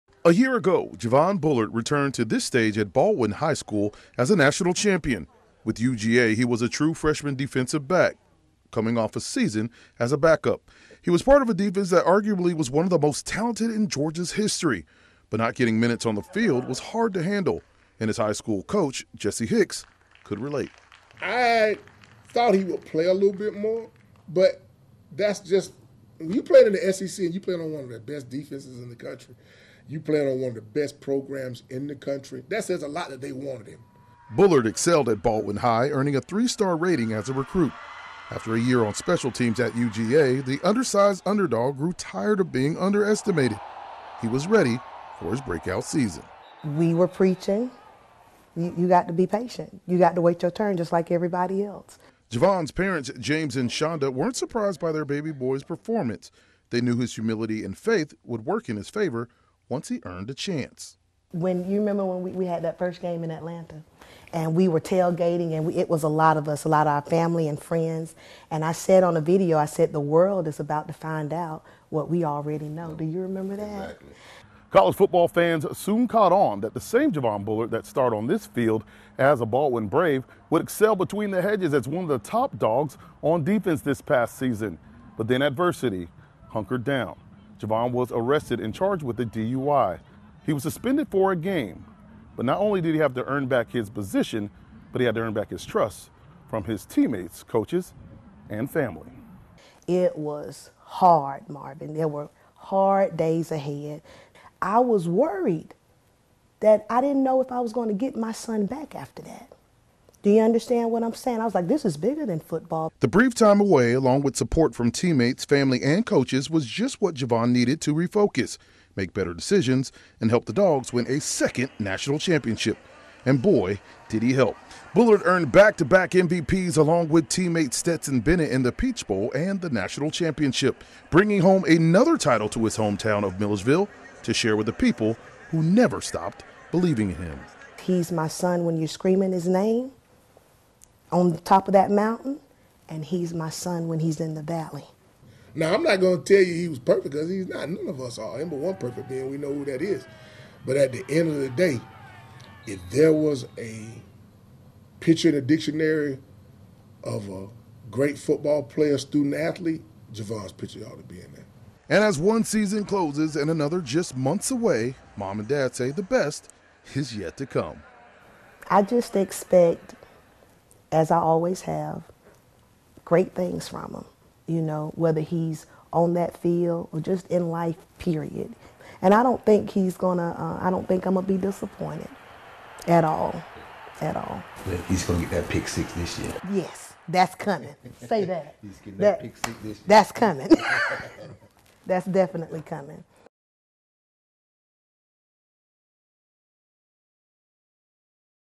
UGA’s Javon Bullard #22
From backup to breakthrough: Milledgeville's Javon Bullard journey to stardom with Georgia Bulldogs - Bullard alongside Stetson Bennett took home a pair of MVP awards in the Peach Bowl and the national championship. via @13WMAZNEWS